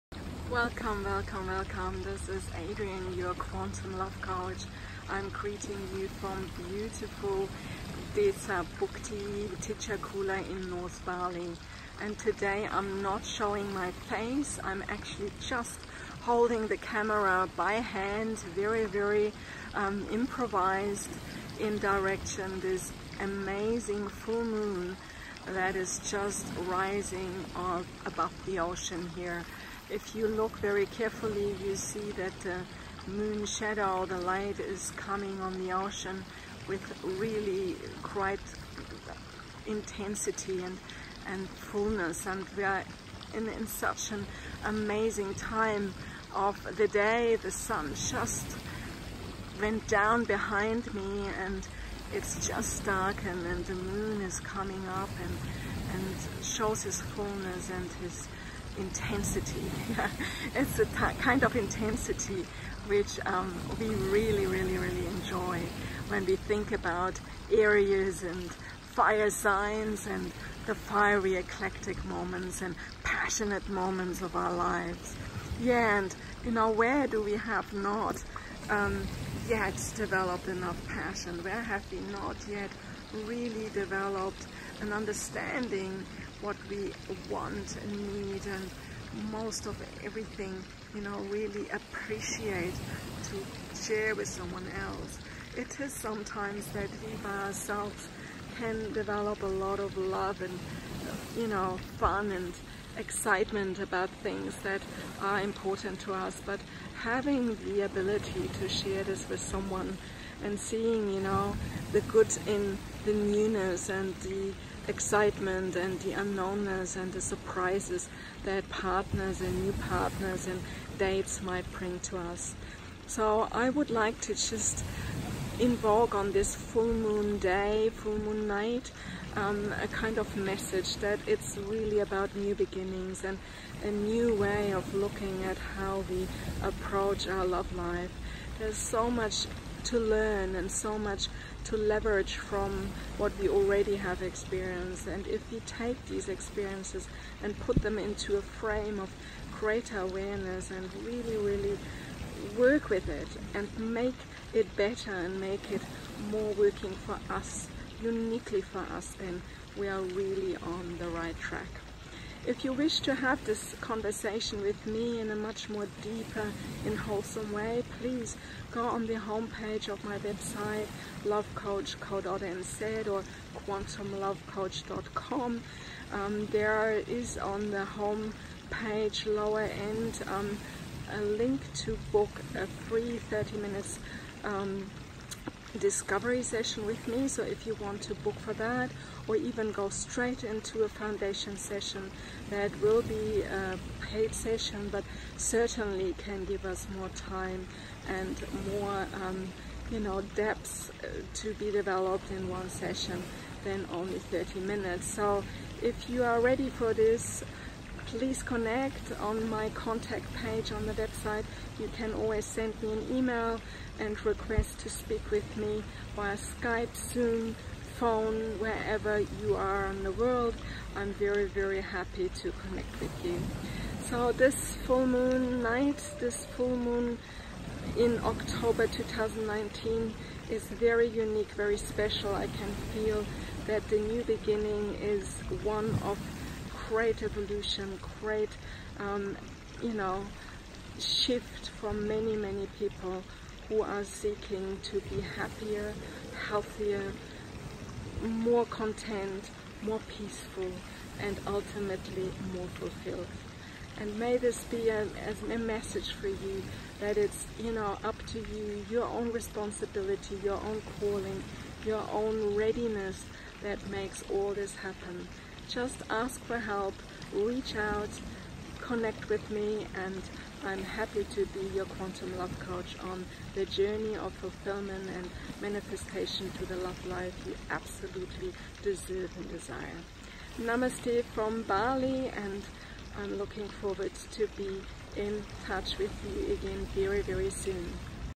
'New Beginnings in our Love Lives' - a QUANTUM LOVE COACH Full Moon in Aries 2019 Message from Bali
Namaste’ and happy Full Moon in Aries on 14th October 2019 - with Greetings from my current home and coaching space, our ‘Love Goddess Villa’ in Tejakula in North Bali!